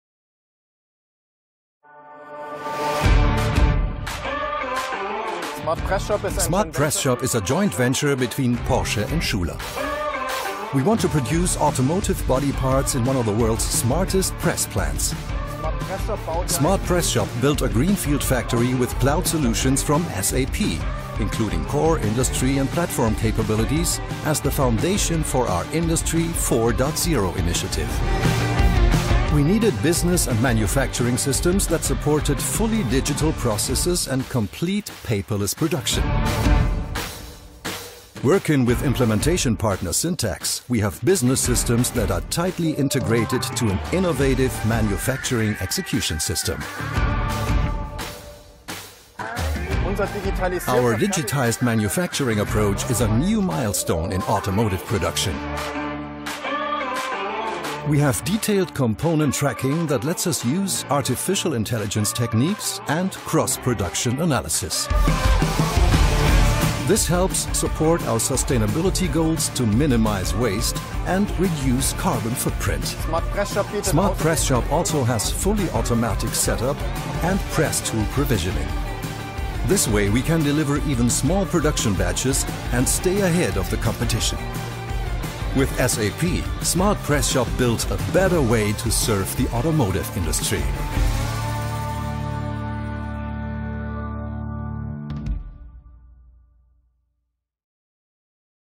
スマートプレスショップ社：中堅自動車部品サプライヤーの経営管理とインダストリー 4.0 に則したスマートファクトリーの実現
スマートプレスショップ社は、ERP と MES を含む SAP のクラウドソリューションにより自動車のボディプレス部品の完全なペーパーレスおよび Industry 4.0 に則したデジタル生産を実現し、収益力と変化対応力の高度化を達成しています。